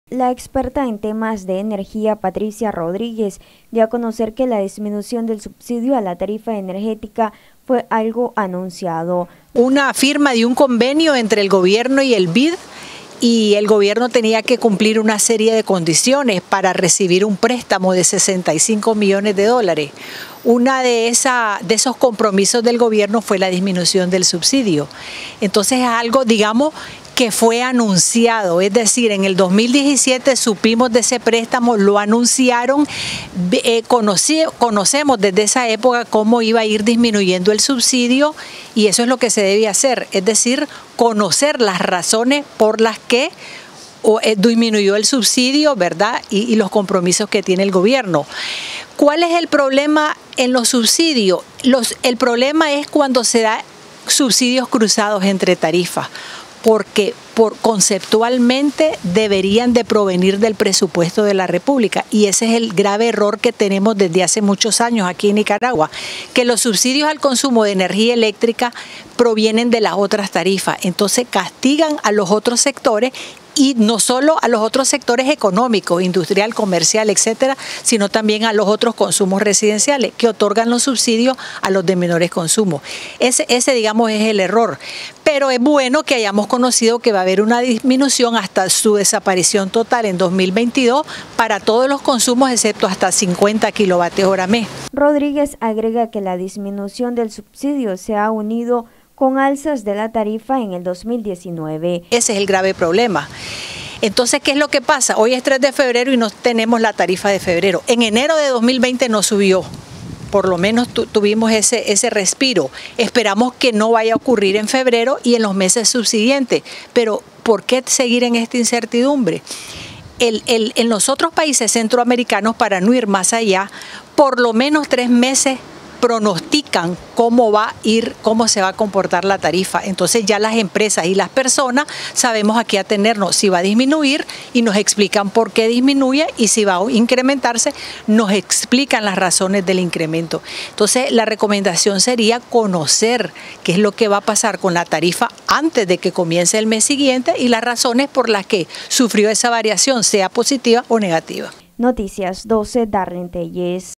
Rodríguez: “La disminución del subsidio a la tarifa energética fue algo anunciado”
La experta en temas de energía dio a conocer que la disminución del subsidio a la tarifa energética fue algo anunciado.